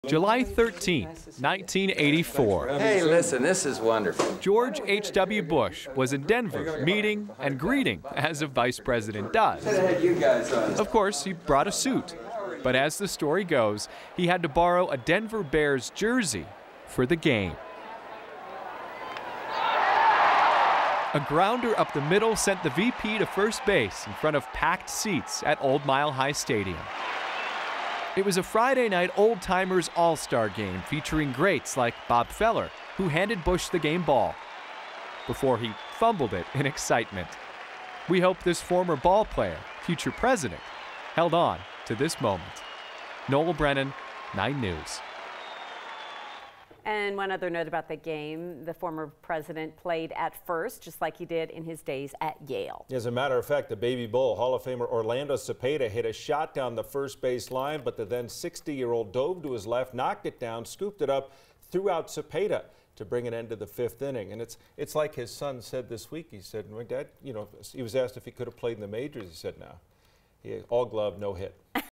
The time George H.W. Bush suited up as a Denver Bear
In 1984, then-Vice President George H.W. Bush suited up in a Denver Bears uniform for an exhibition game at Mile High Stadium featuring some of the game's all-time greats. Bush hit a grounder up the center for a single. As the then 60-year-old vice president jogged off the field, he was given the game ball.

9NEWS (KUSA) is located in Denver, Colorado.